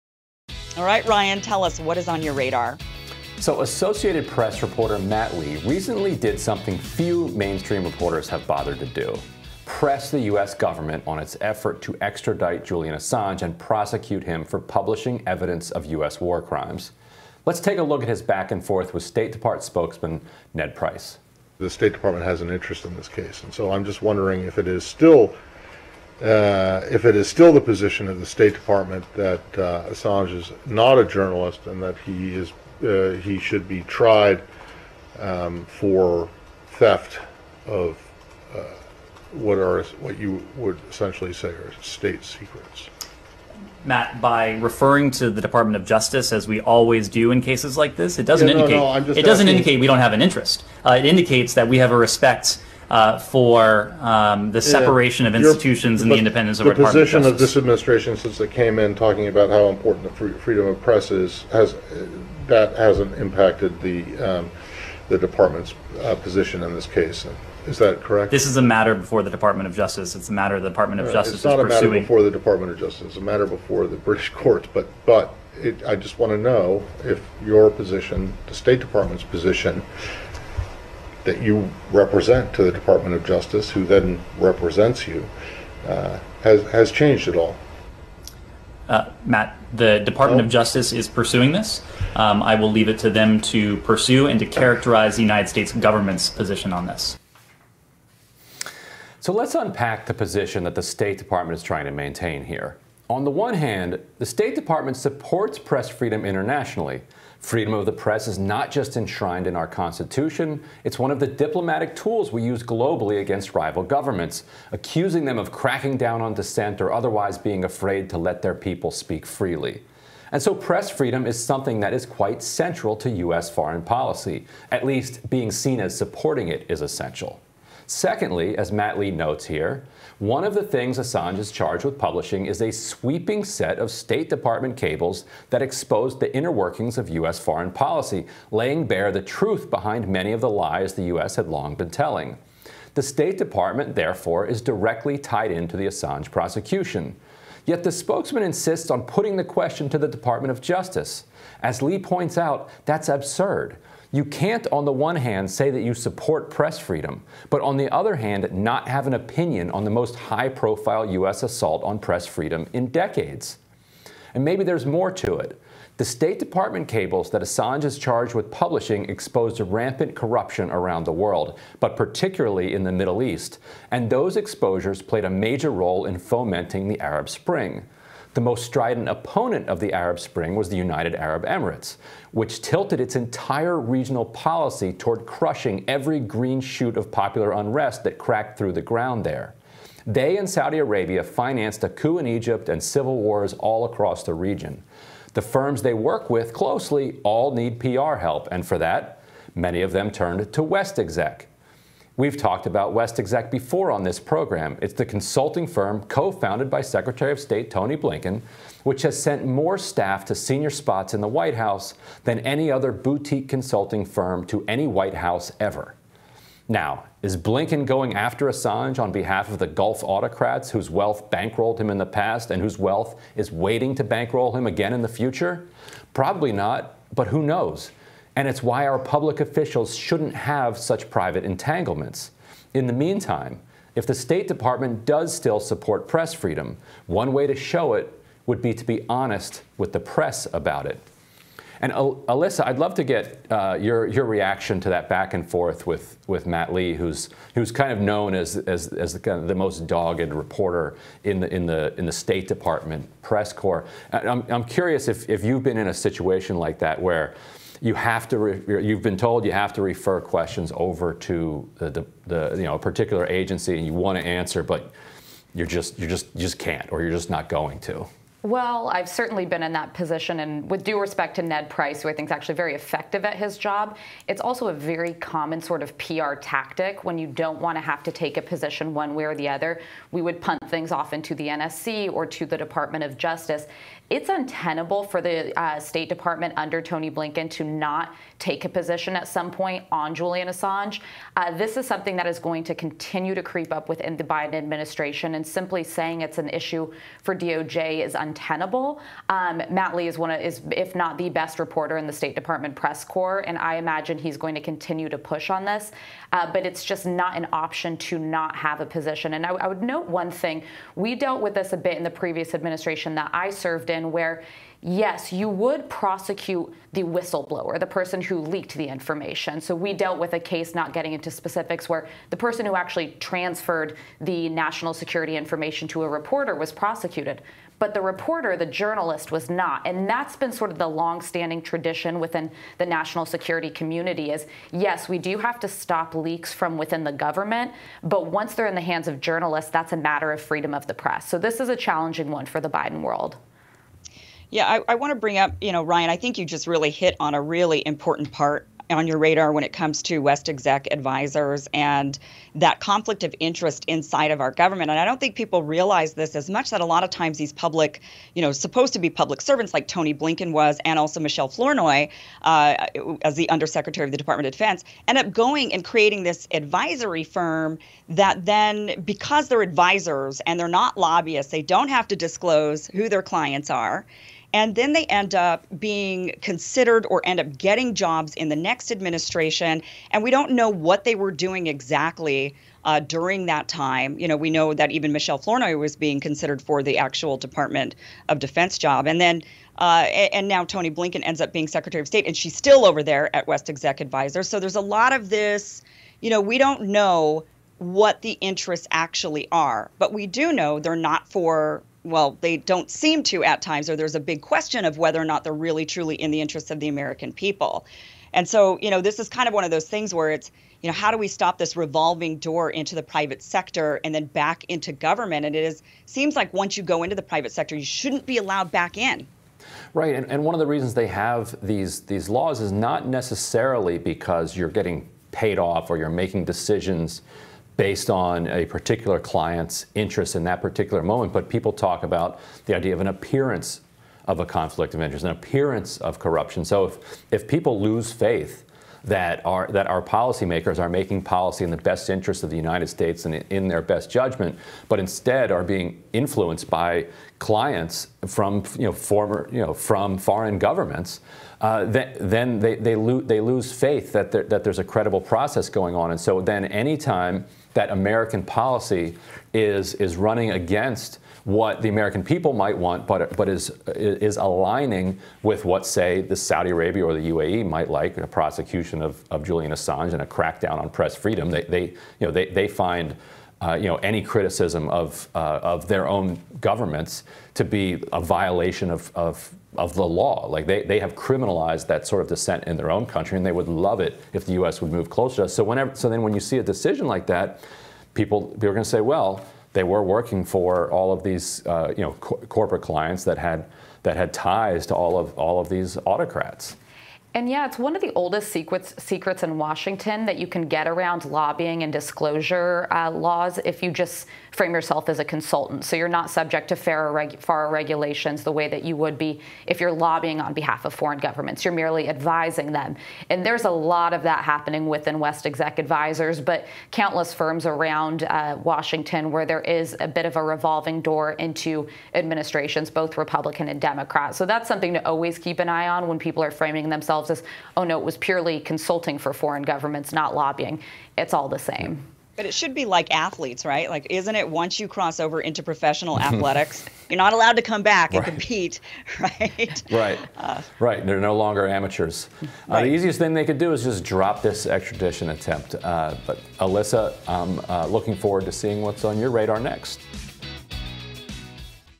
Ryan Grim: State Dept DODGES Question On Julian Assange, Support Of Free Press
Ryan Grim breaks down the state department's hypocritical support of international press freedom while attempting to prosecute Julian Assange.